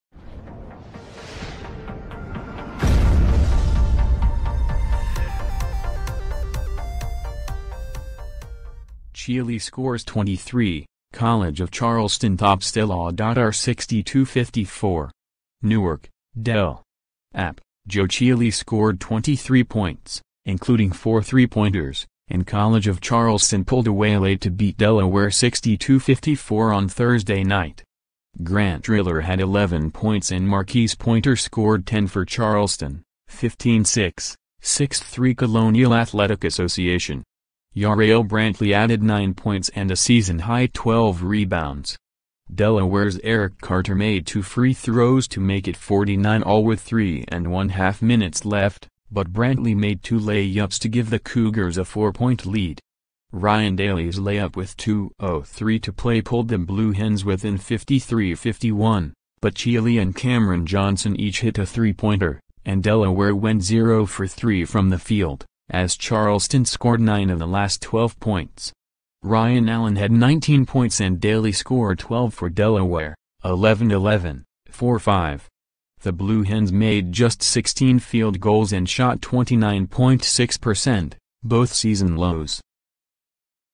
Chealey scores 23, College of Charleston tops Delaw are 62 54
NEWARK, Del. (AP) — Joe Chealey scored 23 points, including four 3-pointers, and College of Charleston pulled away late to beat Delaware 62-54 on Thursday night.

Grant Riller had 11 points and Marquise Pointer scored 10 for Charleston (15-6, 6-3 Colonial Athletic Association). Jarrell Brantley added nine points and a season-high 12 rebounds.

Delaware's Eric Carter made two free throws to make it 49-all with 3½ minutes left, but Brantley made two layups to give the Cougars a four-point lead. Ryan Daly's layup with 2:03 to play pulled the Blue Hens within 53-51, but Chealey and Cameron Johnson each hit a 3-pointer, and Delaware went 0 for 3 from the field, as Charleston scored nine of the last 12 points.

Ryan Allen had 19 points and Daly scored 12 for Delaware (11-11, 4-5). The Blue Hens made just 16 field goals and shot 29.6 percent — both season lows.